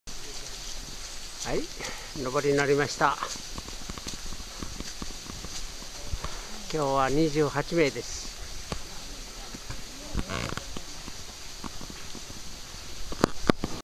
登山
山口県岩国市蓮華山，登る途中負傷したイノシシに遭遇，県警に110番地元猟友会が処分したとの，お知らせがあった。